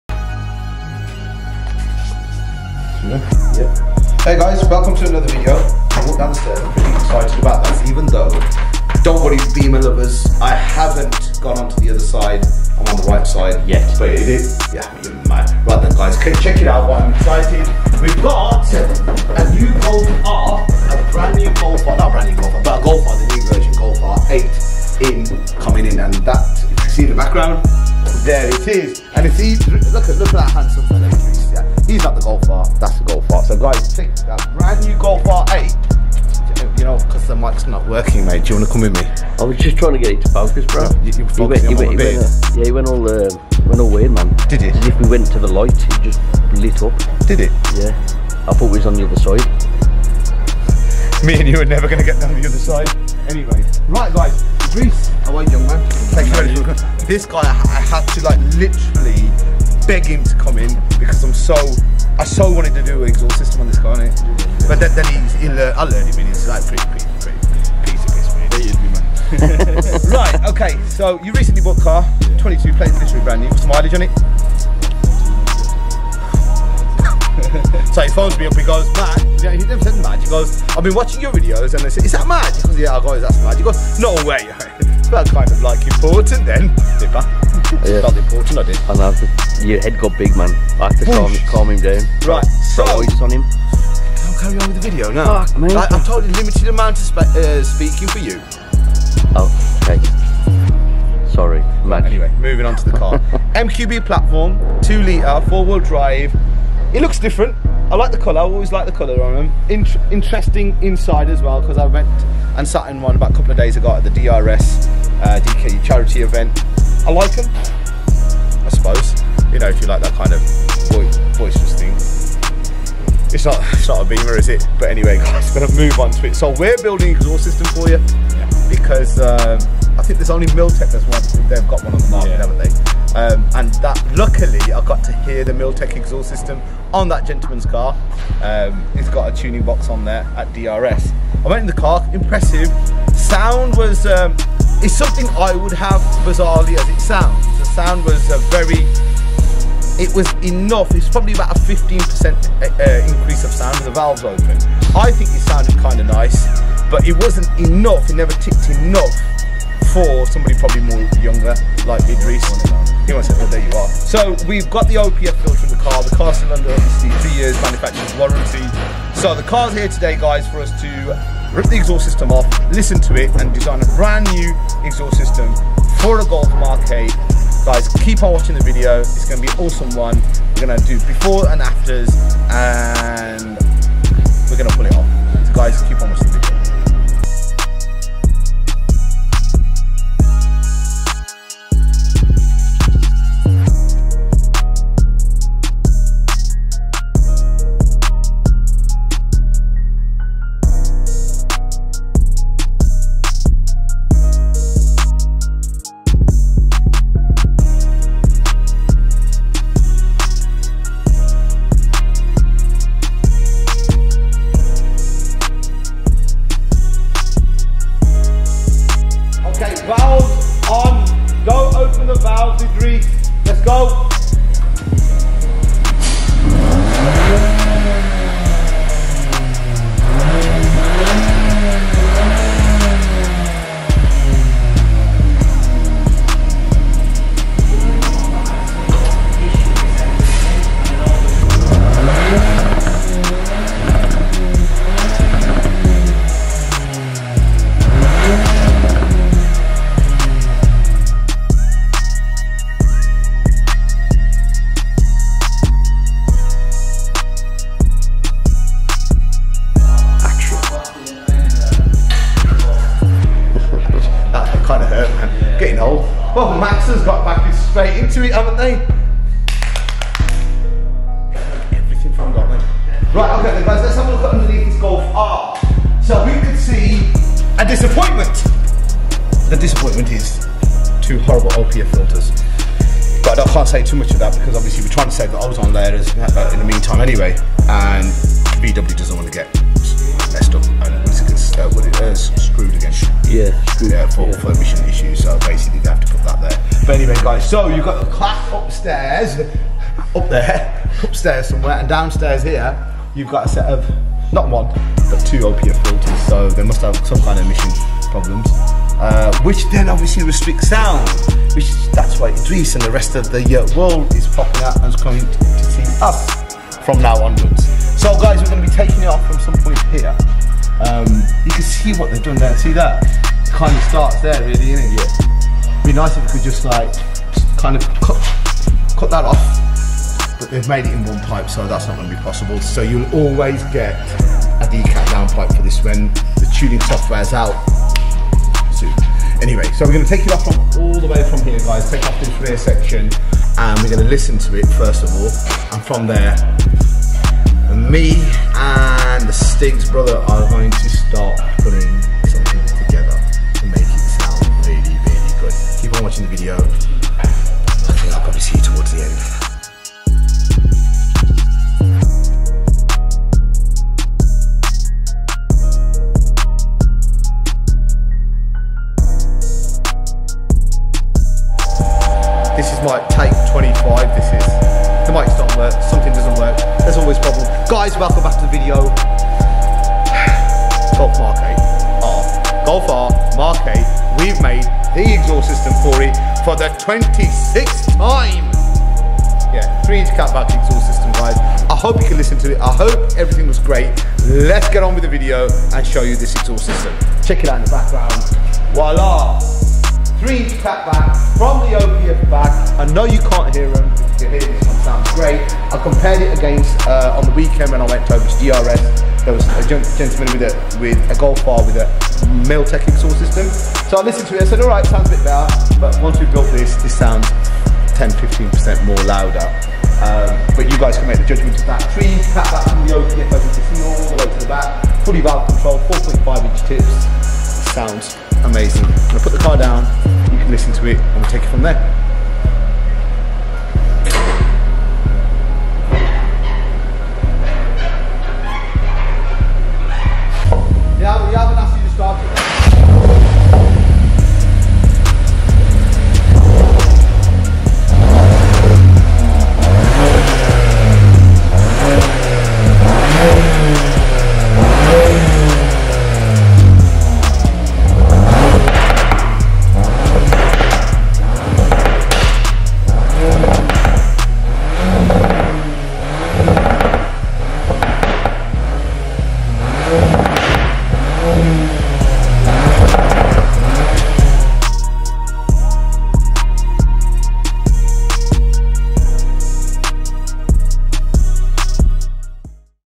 CRACKLES POPS !!! VW GOLF R MK8 VALVE CONTROL OPF BACK PERFORMANCE EXHAUST
Check out this VW GOLF R MK8 VALVE CONTROL performance exhaust custom designed at DKU performance exhaust.

Want to find out more about our various systems? such as the turbo back exhaust, let us know in the comments about any other aspects of the systems you would like to find out about!